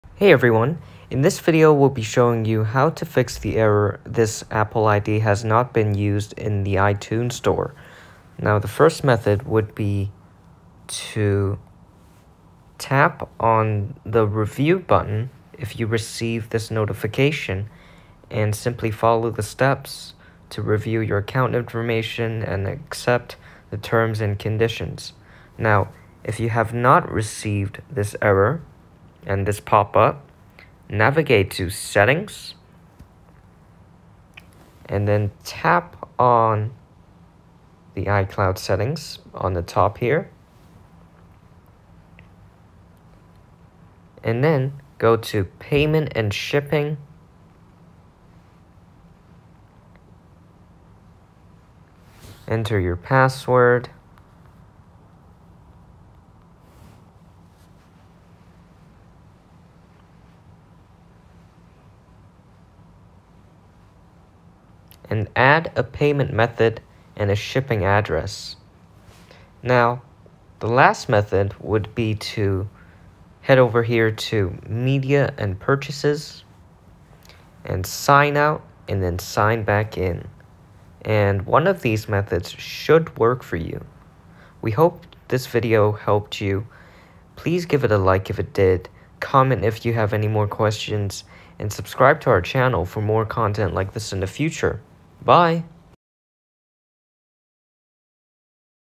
How To FIX This Apple ID Hasn't Been Used In The iTunes Store
Learn How To FIX This Apple ID Hasn't Been Used In The iTunes Store

In this video I will show you how to fix apple ID hasn't been used in your iTunes store